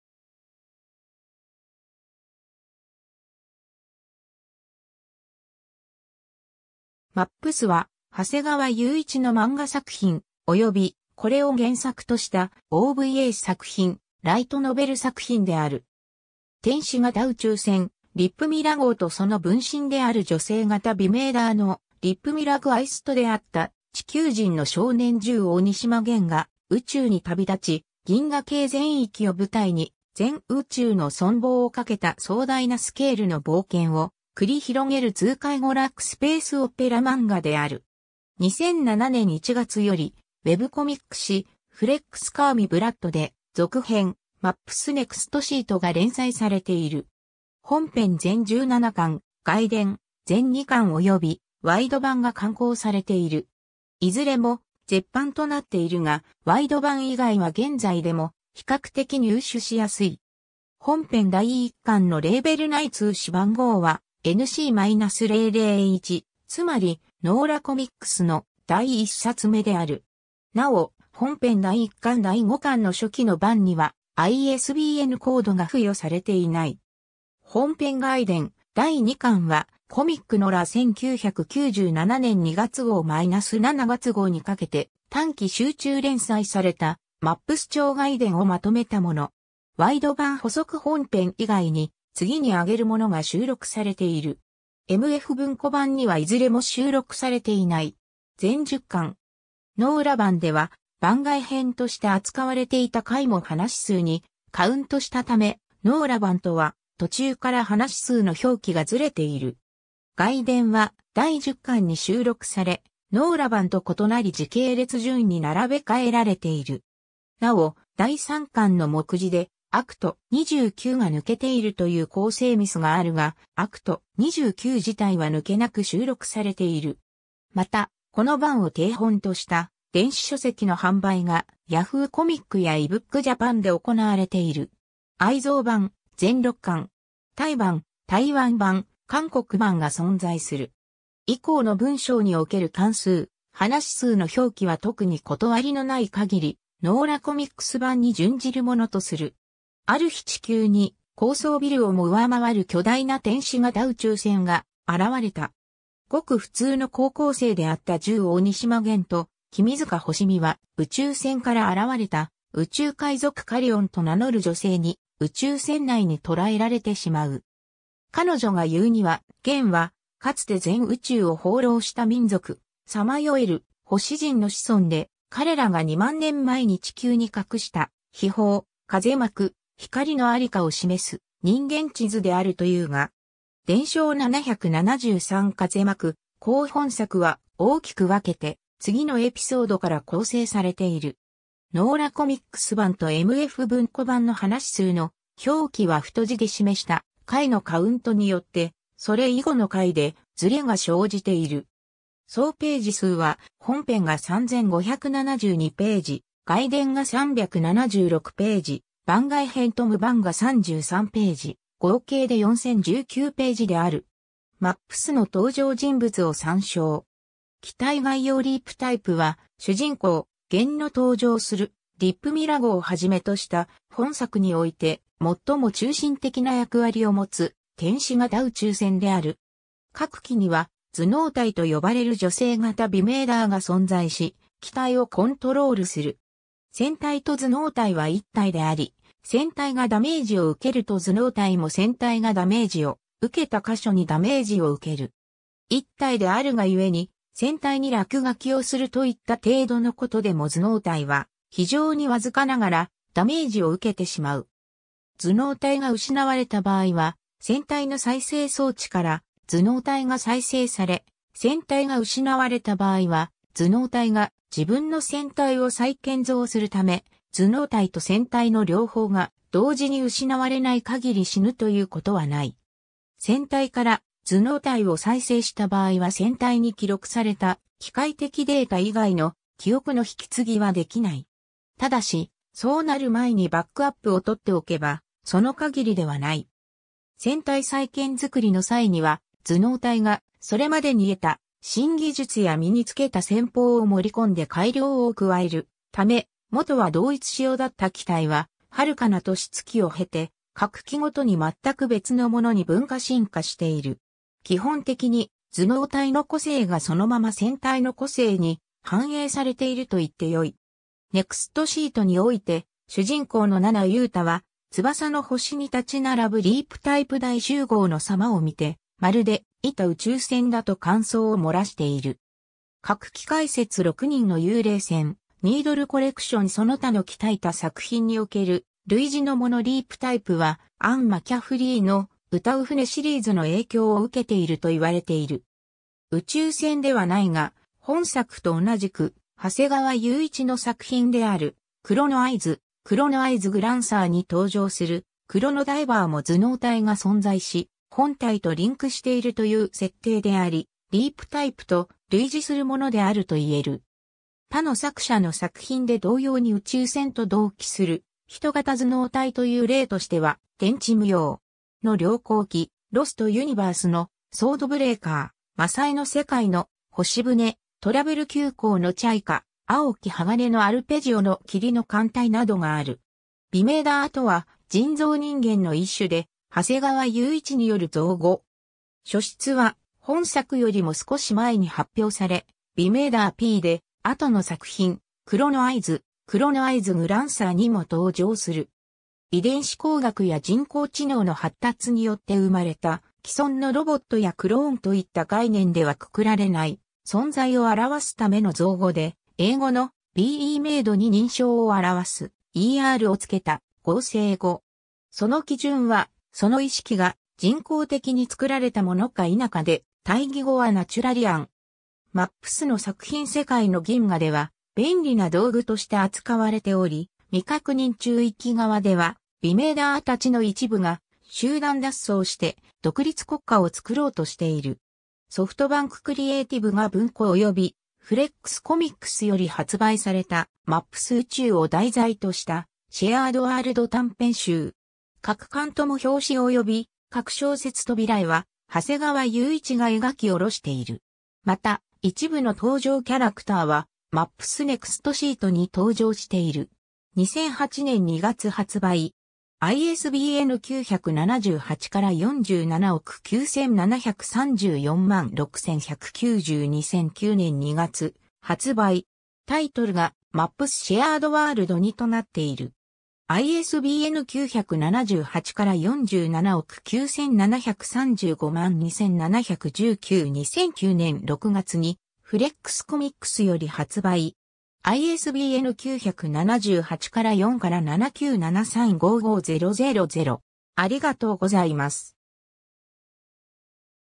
マップス
『マップス』（英: MAPS）は、長谷川裕一の漫画作品、およびこれを原作としたOVA作品（2作品存在する）、ライトノベル作品である。
 天使型宇宙船リプミラ号とその分身である女性型ビメイダー（人造人間の一種で長谷川裕一による造語）のリプミラ・グァイスと出会った地球人の少年十鬼島ゲンが宇宙に旅立ち、銀河系全域を舞台に全宇宙の存亡を賭けた壮大なスケールの冒険を繰り広げる痛快娯楽スペースオペラ漫画である。
 2007年1月よりウェブコミック誌「FlexComixブラッド」で続編『マップス ネクストシート』が連載されている。
 本編全17巻・外伝全2巻およびワイド版が刊行されている。
いずれも絶版となっているが、ワイド版以外は現在でも比較的入手しやすい。
本編第1巻のレーベル内通し番号は「NC-001」、つまりノーラコミックスの第1冊目である。
なお、本編第1巻-第5巻の初期の版にはISBNコードが付与されていない（制度開始前に刊行されたものであるため）。
 本編 外伝 第2巻は「コミックNORA」1997年2月号-7月号にかけて短期集中連載された『マップス超外伝』をまとめたもの。
 ワイド版 補足 本編以外に、次に挙げるものが収録されている。
MF文庫版にはいずれも収録されていない。
 全10巻（初期の版では「外伝」の文庫化が構想外であったために「全9巻」と表記されている）。
ノーラ版では番外編として扱われていた回も話数にカウントしたため、ノーラ版とは途中から話数の表記がずれている（詳しくはサブタイトルの節を参照のこと）。
外伝は第10巻に収録され、ノーラ版と異なり時系列順に並べ替えられている。
なお、第3巻の目次でACT.29が抜けているという校正ミスがあるがACT.29自体は抜けなく収録されている。
 また、この版を底本とした電子書籍の販売がYahoo!コミックやeBookJapanで行われている。
 愛蔵版、全6巻。
 タイ版、台湾版、韓国版が存在する。
 以降の文章における巻数・話数の表記は特に断りのない限りノーラコミックス版に準じるものとする。
 ある日地球に高層ビルをも上回る巨大な天使型宇宙船が現れた。
ごく普通の高校生であった十鬼島ゲン（ときしま げん）と君塚星見（きみずか ほしみ）は宇宙船から現れた宇宙海賊カリオンと名乗る女性に宇宙船内に捕らえられてしまう。
彼女が言うにはゲンはかつて全宇宙を放浪した民族「さまよえる星人（さまよえるほしびと）」の子孫で、彼らが2万年前に地球に隠した秘宝「風まく光」のありかを示す人間地図（マップマン）であるというが…。
 伝承773 風まく光 本作は、大きく分けて次のエピソードから構成されている。
 ノーラコミックス版とMF文庫版の話数の表記は太字で示した回のカウントによって、それ以後の回でずれが生じている。
総ページ数は本編が3572ページ・外伝が376ページ・番外編と無番が33ページ、合計で4019ページである。
 マップスの登場人物を参照。
 機体概要 リープタイプ (REEPTYPE) は、主人公・ゲンの搭乗するリプミラ号をはじめとした、本作において最も中心的な役割をもつ天使型宇宙船である。
各機には頭脳体と呼ばれる女性型ビメイダーが存在し、機体をコントロールする。
船体と頭脳体は一体であり、船体がダメージを受けると頭脳体も船体がダメージを受けた箇所にダメージを受ける（ただし、あくまでもダメージを感じるだけであり、致死のダメージまでは影響しない）。
一体であるがゆえに「船体にラクガキをする」といった程度のことでも頭脳体は非常にわずかながらダメージを受けてしまう（ダメージと言うよりはくすぐったい）。
 頭脳体が失われた場合は船体の再生装置から頭脳体が再生され、船体が失われた場合は頭脳体が自分の船体を再建造するため、頭脳体と船体の両方が同時に失われない限り死ぬ（消滅する）ということは無い。
船体から頭脳体を再生した場合は船体に記録された機械的データ以外の「記憶」の引継ぎはできない。
ただし、そうなる前にバックアップを取っておけば、その限りではない。
 船体再建造の際には、頭脳体がそれまでに得た新技術や身に付けた戦法を盛り込んで改良を加えるため、元は同一仕様だった機体は遥かな年月を経て各機ごとに全く別のものに分化進化している。
基本的に頭脳体の個性（性格）がそのまま船体の個性（能力）に反映されているといっ...